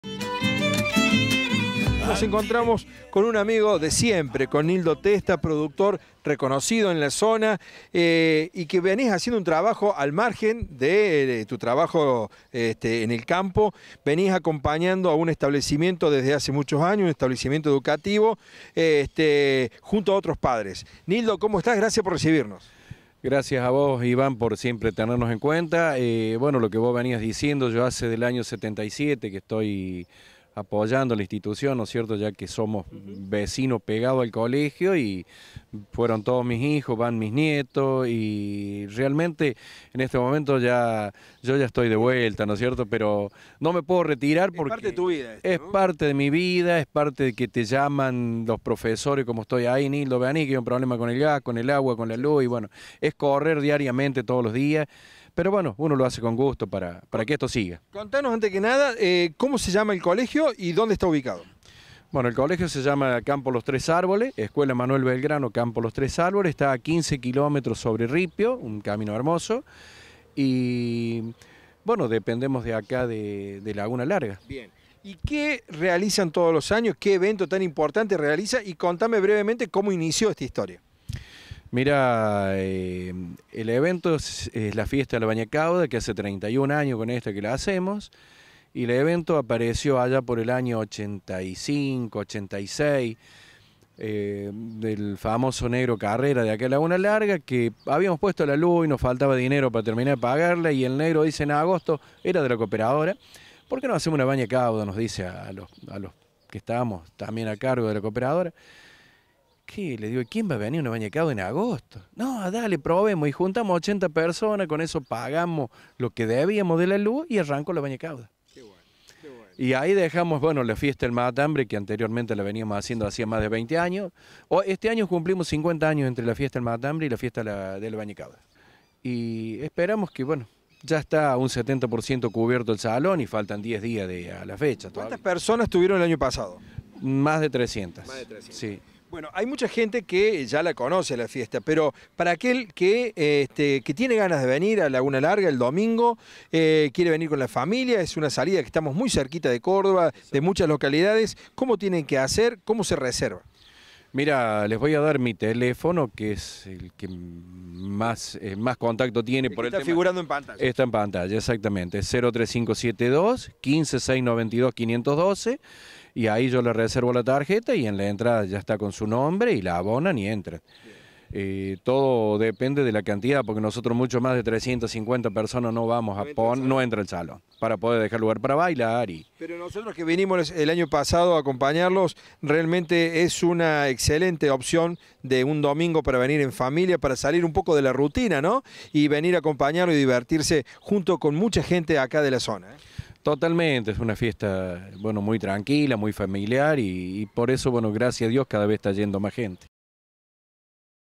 Fiesta de la Bagna Cauda, Campo Los tres árboles
El 25 de junio próximo se realizará en Campo Los Tres Árboles, Laguna Larga, la tradicional bagna cauda, organizada por la Cooperadora Juan Bautista Alberdi.
Como siempre, lo recaudado es a beneficio del IPEM 342 Dr. René Favaloro, anexo Los Tres Árboles. Actuará el Cuarteto Juventud.